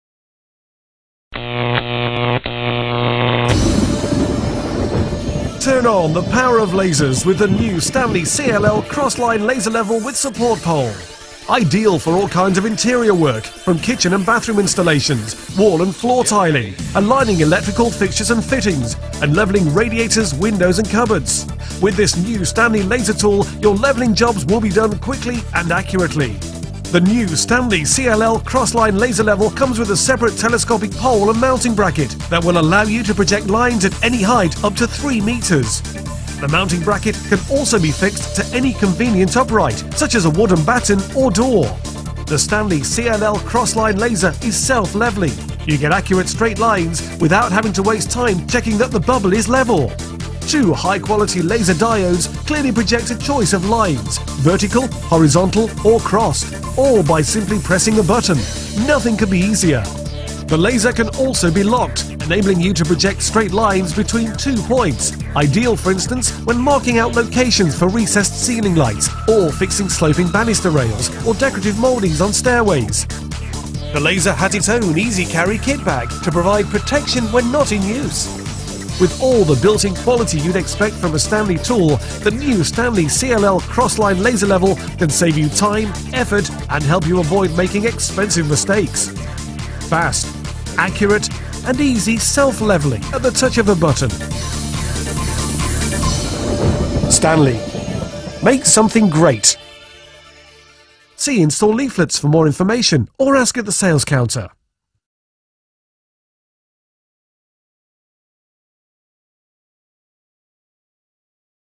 Stanley Automatische Mini Kruislaser CLL 77-214
De automatische, compacte en bijzonder gebruiksvriendelijke CLL is ideaal voor het aanleggen van electrische installaties en overige binnenwerkzaamheden zoals tegels leggen, lampen bevestigen en binnenhuisinrichting.

Met één druk op de knop projecteert u zowel verticale als horizontale lijnen, of een kruis. Dankzij de automatische waterpasinstelling kunt u makkelijk hellingen uittekenen.

- Automatische waterpasinstelling met magnetisch compensatiesysteem
- Systeem waarschuwt indien buiten automatisch waterpasinstellingsbereik

Kenmerken:

- Diode Laser: 635 nm - Klasse 2M
- Nauwkeurigheid: 6 tot 9 mm
- Nivelleerbereik: plus of min 5 °
- Bereik: tot 10 meter binnenshuis
- Statiefschroefdraad 1 / 4 "
- Gevoed door 3 AA alkaline batterijen LR6

Inhoud: Automatische Laser cross CLL, telescopisch statief, basis met schroefdraad 1/4", draagtas, batterijen